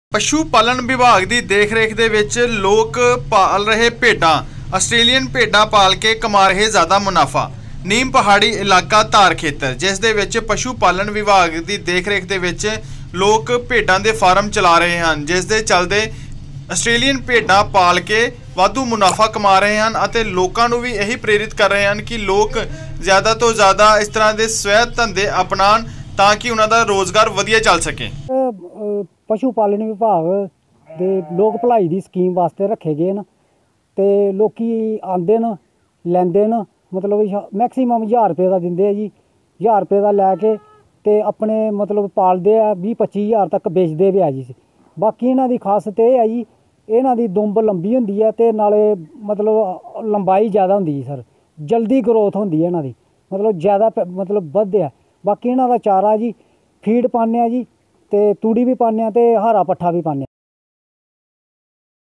ਅਸਟ੍ਰੇਲੀਅਨ ਭੇਡਾਂ ਪਾਲ ਕੇ ਚੰਗਾ ਮੁਨਾਫ਼ਾ ਕਮਾ ਰਹੇ ਪਸ਼ੂ ਪਾਲਕ | ETV Bharat Punjab
ਜ਼ਿਲ੍ਹੇ ਦਾ ਨੀਮ ਪਹਾੜੀ ਇਲਾਕਾ ਧਾਰ ਕਲਾਂ 'ਚ ਪਸ਼ੂ ਪਾਲਣ ਵਿਭਾਗ ਵੱਲੋਂ ਖੇਤੀ ਨਾਲ ਜੁੜੇ ਲੋਕਾਂ ਨੂੰ ਰੁਜ਼ਗਾਰ ਦੇਣ ਦੀ ਕੋਸ਼ਿਸ਼ ਕੀਤੀ ਜਾ ਰਹੀ ਹੈ। ਧਾਰ ਕਲਾਂ ਖੇਤਰ 'ਚ ਪੈਂਦੇ ਪਿੰਡ ਡੱਲਾ ਵਿਖੇ ਇੱਕ ਭੇਡ ਫਾਰਮ ਖੋਲ੍ਹਿਆ ਗਿਆ ਹੈ। ਇਥੇ ਧਾਰ ਦੇ ਲੋਕ ਘੱਟ ਲਾਗਤ 'ਚ ਭੇਡ ਖਰੀਦ ਕੇ ਉਨ੍ਹਾਂ ਨੂੰ ਪਾਲ ਕੇ ਚੰਗਾ ਮੁਨਾਫ਼ਾ ਕਮਾ ਸਕਦੇ ਹਨ। ਇਸ ਇਲਾਕੇ ਦੇ ਜ਼ਿਆਦਾਤਰ ਲੋਕ ਖੇਤੀ ਨਾਲ ਸਬੰਧਤ ਛੋਟੇ-ਛੋਟੇ ਕੰਮ ਕਰਦੇ ਹਨ। ਇਸ ਲਈ ਪਸ਼ੂ ਵਿਭਾਗ ਵੱਲੋਂ ਇਥੋਂ ਦੇ ਲੋਕਾਂ ਲਈ ਇਹ ਖ਼ਾਸ ਉਪਰਾਲਾ ਕੀਤਾ ਗਿਆ। ਇਸ ਬਾਰੇ ਦੱਸਦੇ ਹੋਏ ਸਥਾਨਕ ਪਸ਼ੂ ਪਾਲਕ ਅਸ਼ੋਕ ਕੁਮਾਰ ਨੇ ਦੱਸਿਆ ਕਿ ਉਸ ਨੇ ਵਿਭਾਗ ਦੀ ਦੇਖ ਰੇਖ 'ਚ ਆਪਣਾ ਭੇਡ ਫਾਰਮ ਚਲਾਇਆ। ਹੁਣ ਉਹ ਅਸਟ੍ਰੇਲੀਅਨ ਭੇਡਾਂ ਪਾਲ ਕੇ ਚੰਗਾ ਮੁਨਾਫ਼ਾ ਕਮਾ ਰਿਹਾ ਹੈ। ਉਸ ਨੇ ਖੇਤਰ ਦੇ ਹੋਰਨਾਂ ਕਿਸਾਨਾਂ ਨੂੰ ਇਸ ਯੋਜਨਾ ਦਾ ਲਾਭ ਲੈਣ ਦੀ ਅਪੀਲ ਕੀਤੀ ਤਾਂ ਜੋ ਕਿਸਾਨਾਂ ਦੀ ਆਮਦਨ 'ਚ ਵਾਧਾ ਹੋ ਸਕੇ।